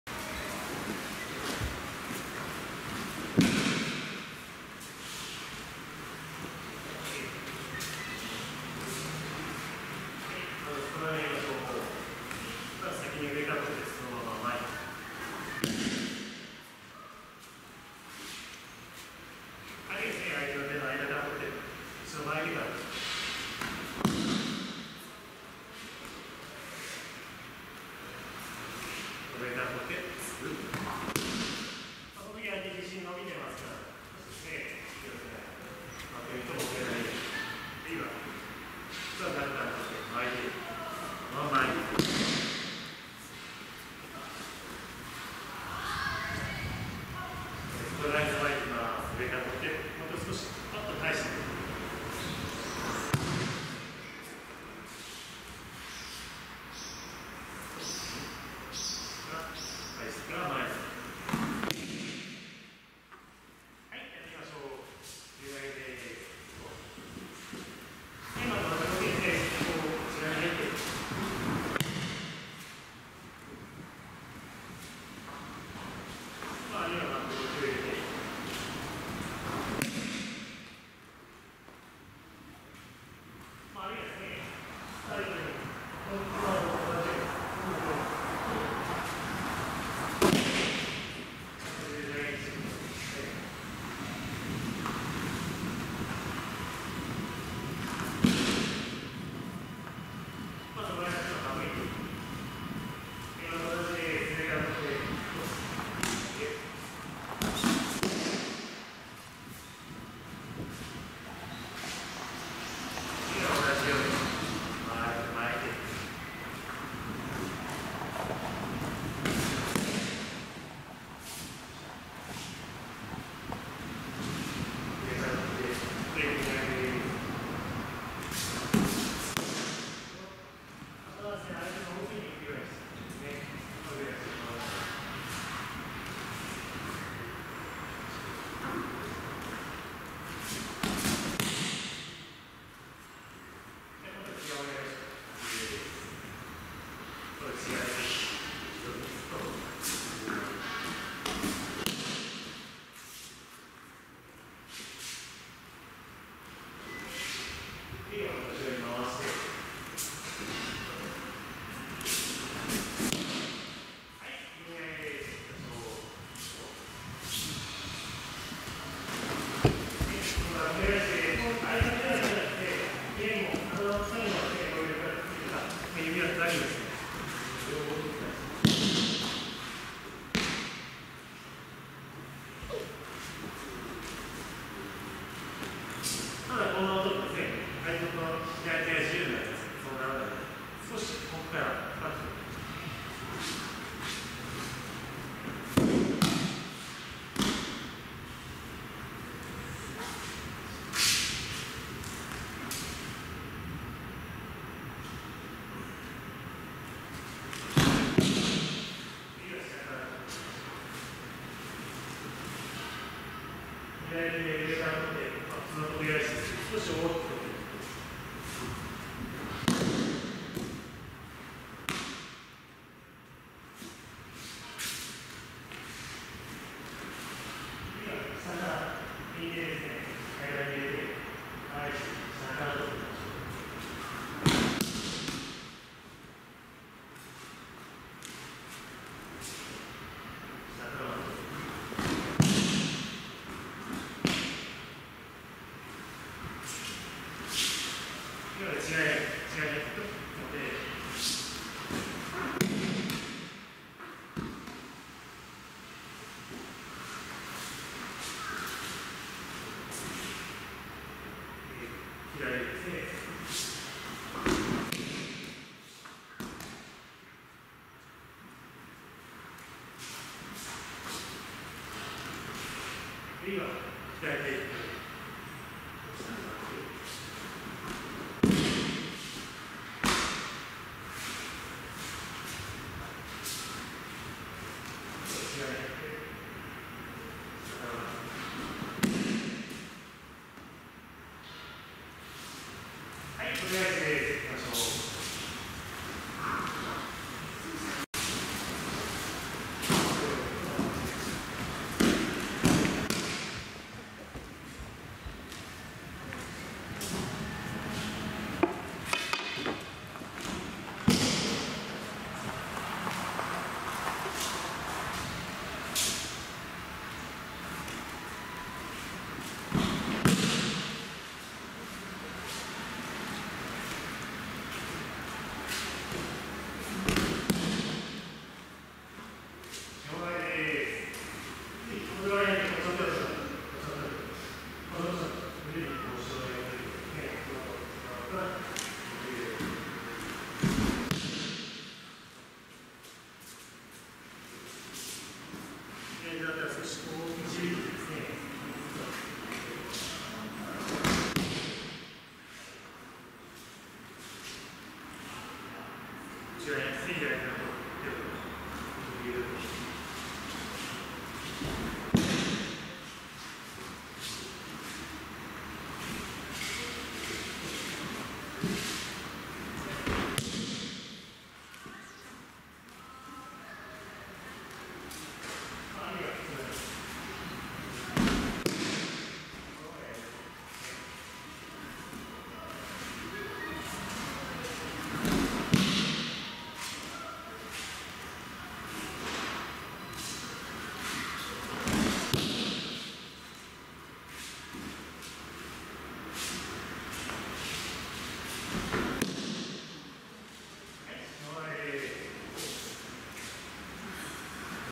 木剣取り呼吸投げ、十字投げ、入身投げ、小手返し、四方投げ 合気道入江道場研鑽会 2017年10月
合気道 入江道場 Aikido Irie dojo Aikikai Kyoto